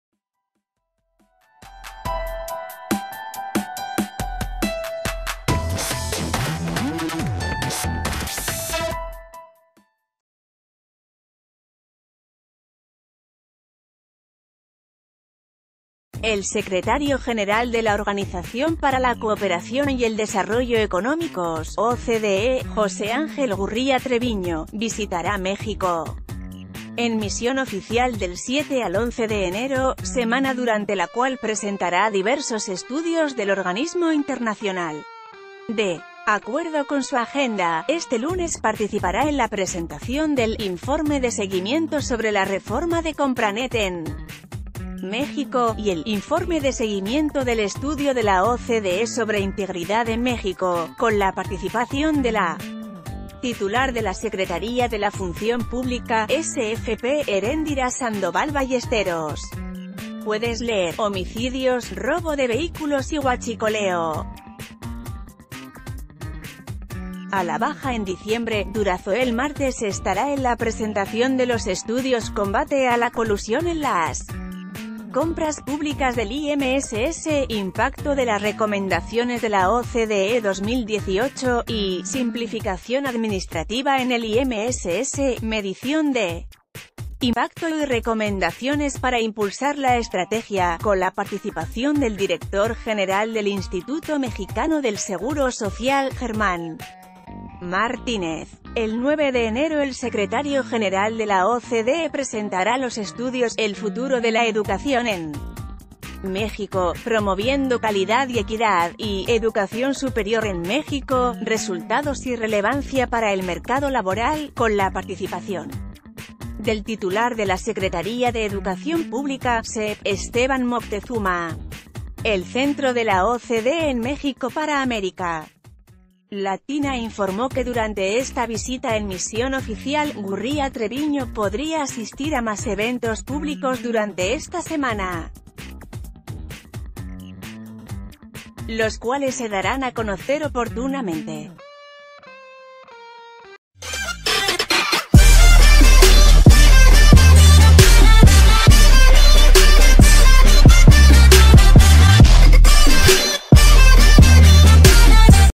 OCDE presentará informe sobre inseguridad en México
El secretario general de la Organización para la Cooperación y el Desarrollo Económicos (OCDE), José Ángel Gurría Treviño, visitará México en misión oficial del 7 al 11 de enero, semana durante la cual presentará diversos estudios del organismo internacional. De acuerdo con su agenda, este lunes par...